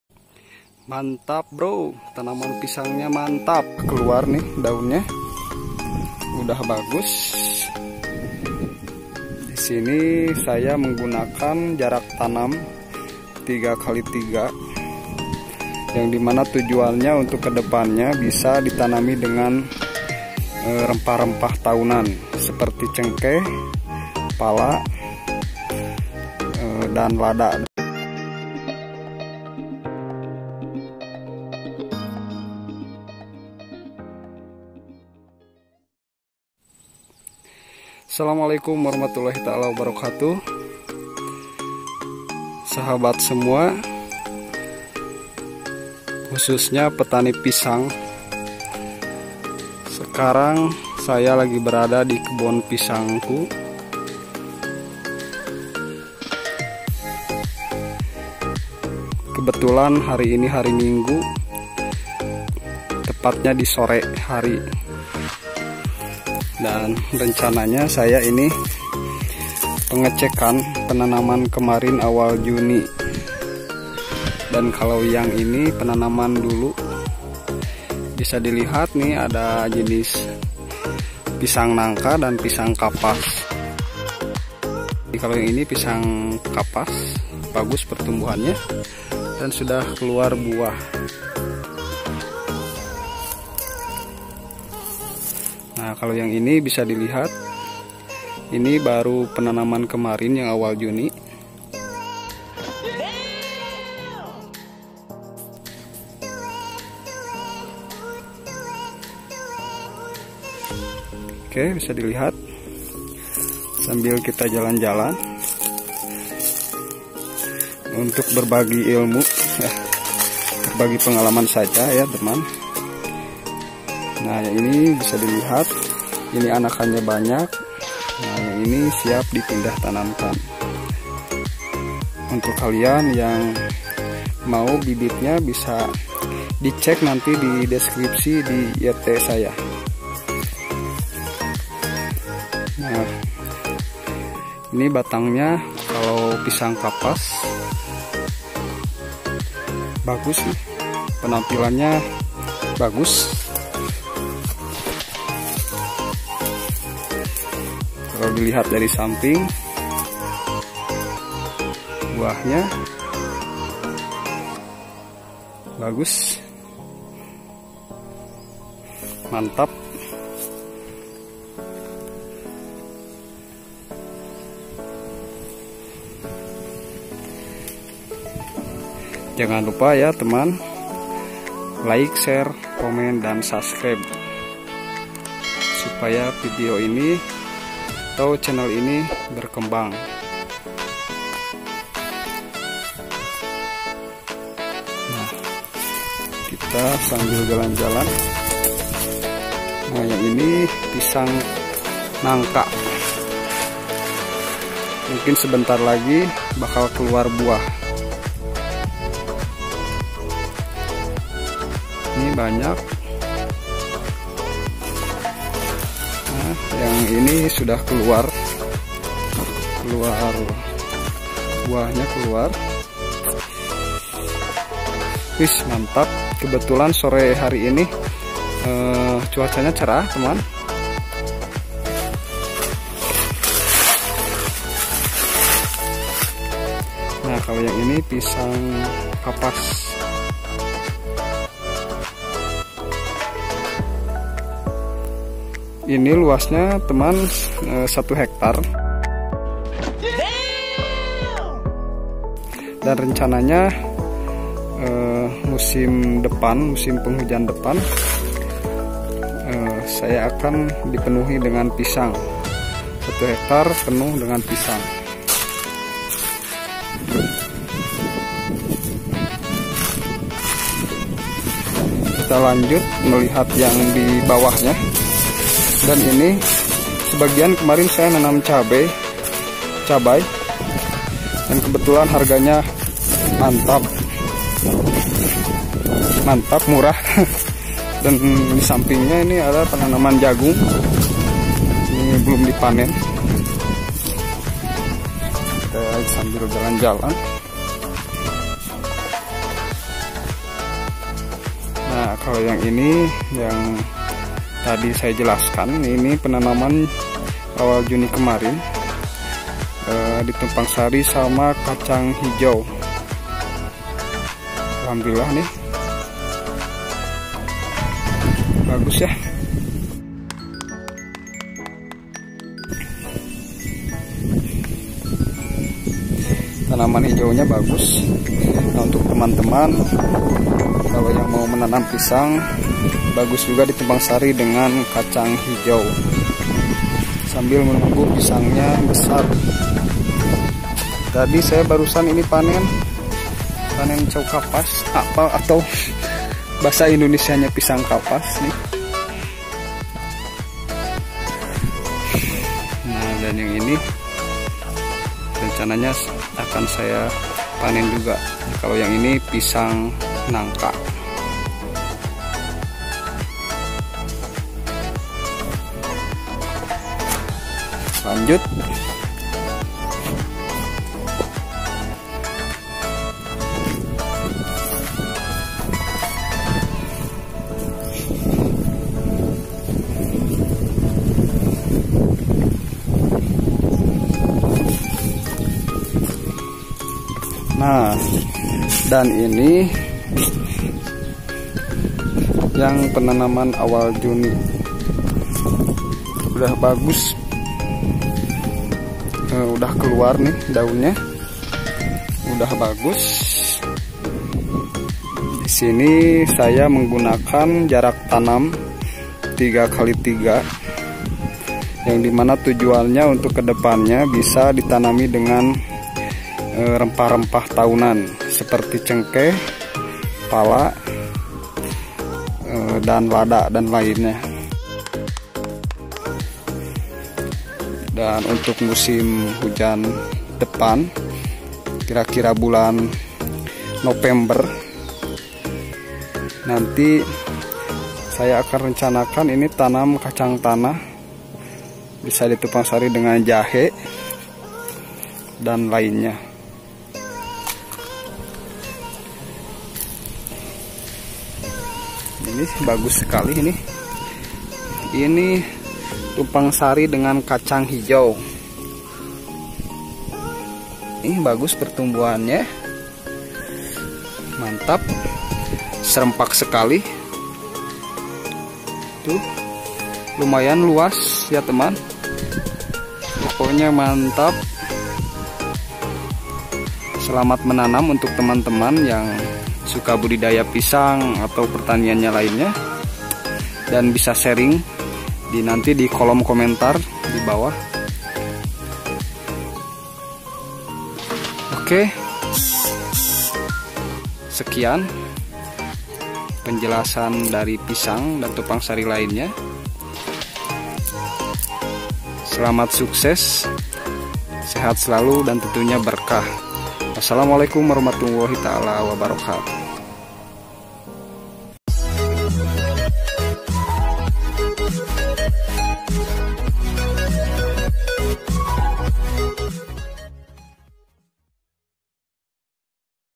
Ternyata mudah menanam pisang ini tanpa modal puluhan juta!
Ini alasannya ! Sektor pertanian makin di gemari generasi muda, terutama di budidaya pisang. Karena dengan harga yang standar sehingga keuntungan lumayan karena mengejar volume yang besar.
Kepada seluruh petani khususnya petani muda ayo kita saatnya budidaya tanaman pisang atau komoditas tanaman apapun sampai ke tanaman tahunan yaitu rempah-rempah.
Supaya kita prekonomian semakin berdiri kuat dan tidak berlihak selalu kepada negara lain.
Jika ada saran atau lainnya tulis dikolom komentar ya sahabat, kita sering saja tentang pertanian supaya maju bersama-sama.

Fb; ace agrofarm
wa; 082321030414